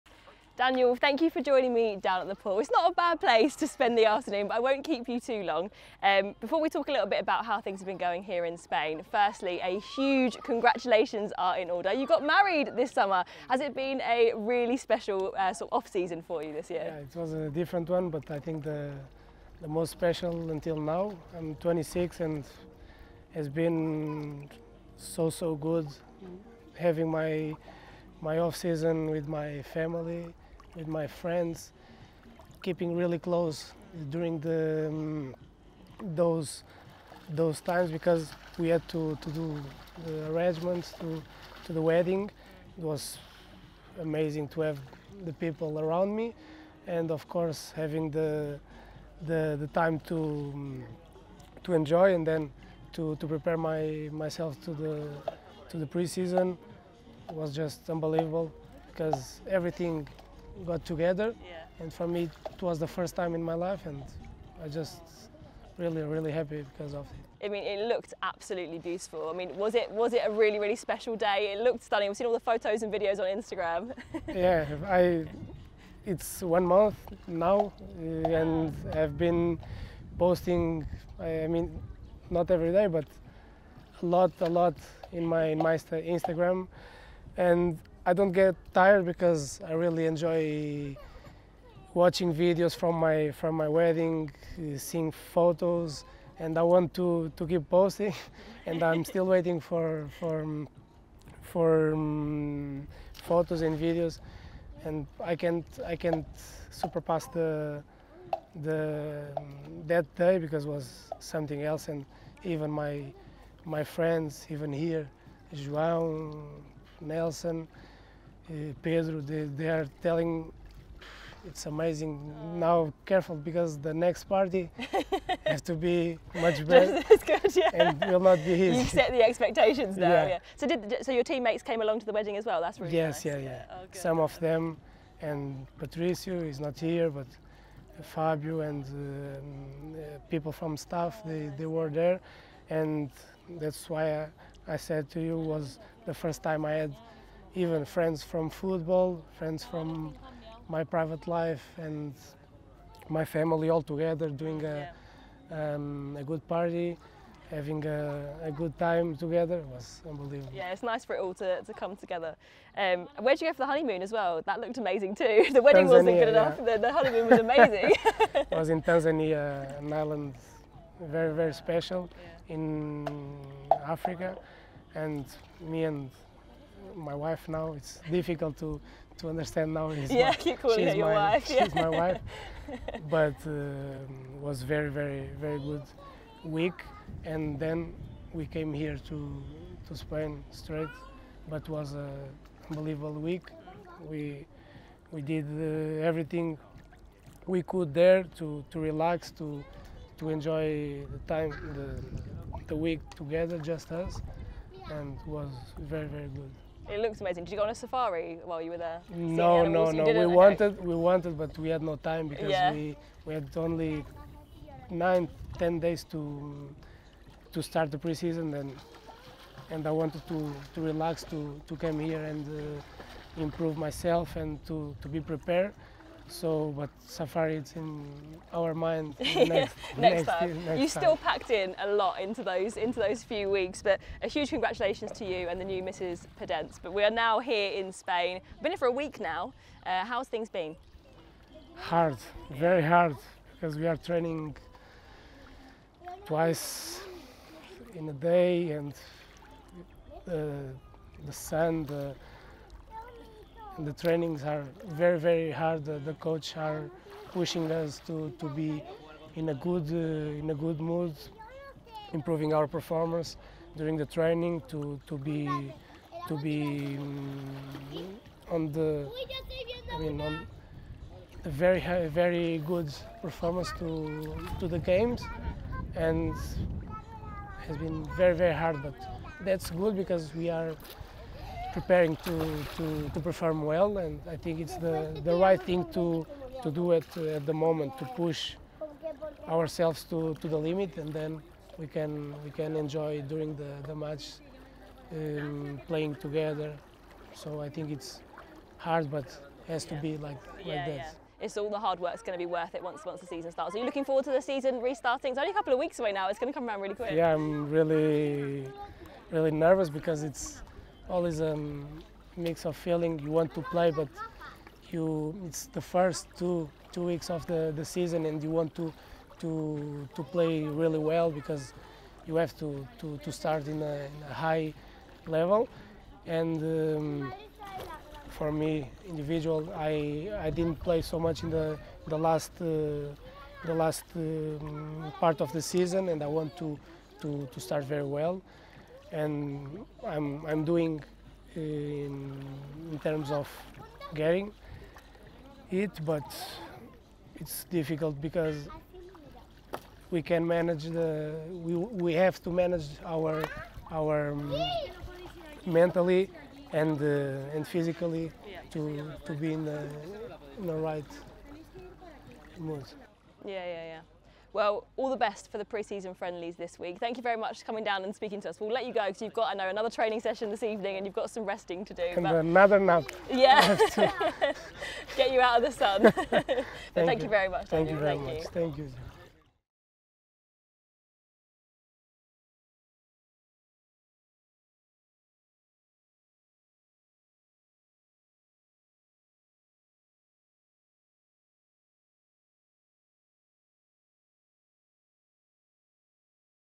Podence on his wedding celebrations, honeymooning in Tanzania and the training camp
Gemma Frith sat down with Daniel Podence after his busy off-season. He talks getting married in Portugal in the company of many of his teammates, heading to Tanzania for his honeymoon and then the double sessions in Alicante as he prepares to have his best season yet.